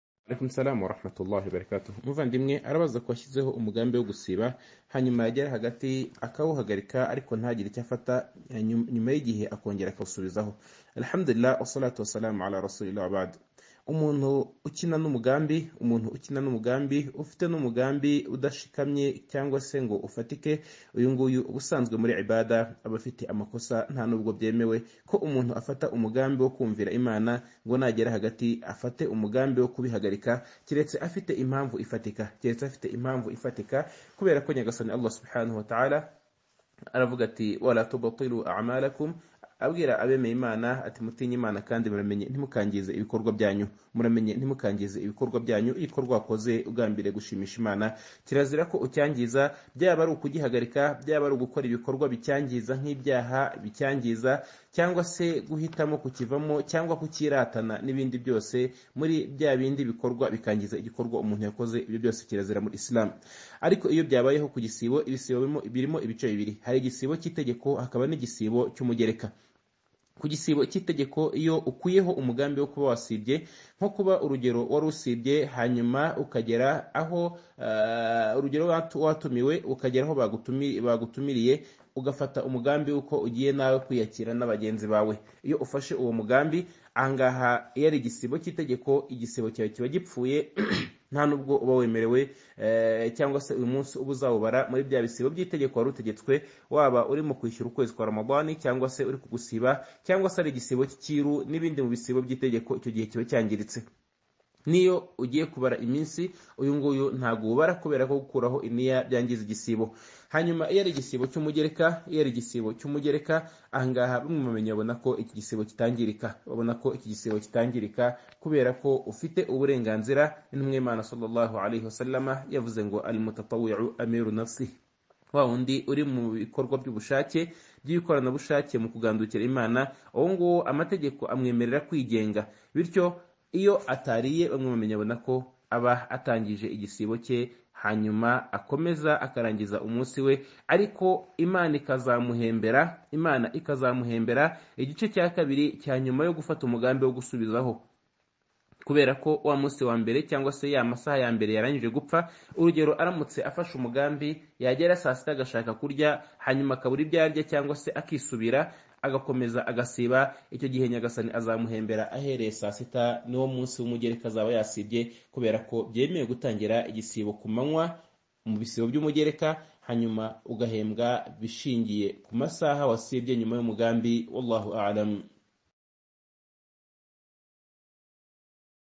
IGISUBIZO KU KIBAZO ~ Episode 1017: usibye RAMADHAN hanyuma agafata umugambi wo kwica umunsi ku mp…👇
Asalam alaikum warahmatullahi wabarakatuh

      *IGISUBIZO KU KIBAZO*

*usibye RAMADHAN hanyuma agafata umugambi wo kwica umunsi ku mpamvu yemewe yarangiza akisubiraho akawukomeza akawusiba?*

Umuvandimwe: Niyitanga Djamidu

*RISANGIZE  ABAVANDIMWE JAZAKA LLAHU KHAIRAN*